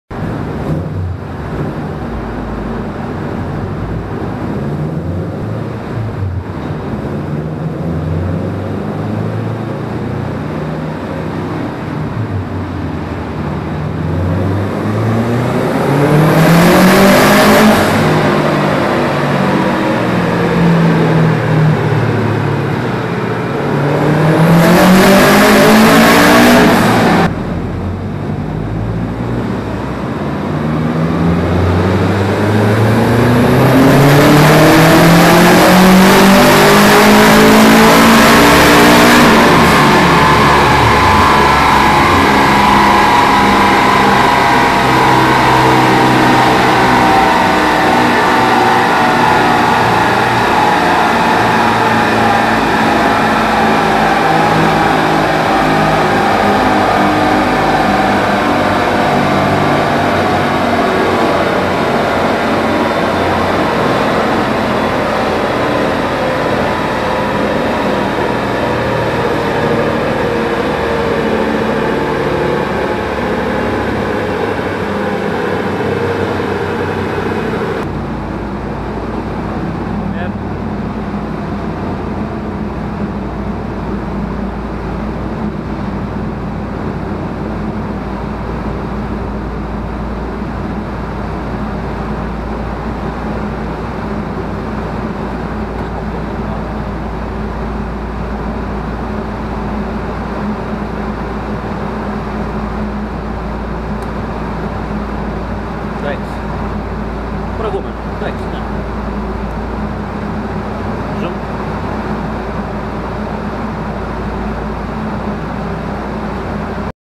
Forester 2.5 Sti Turbo Aps Sr 40
Turbo Aps Sr 40 1.3 overboost 1.1 stable
525 injectors
Walbro 255 intank
Stock ecu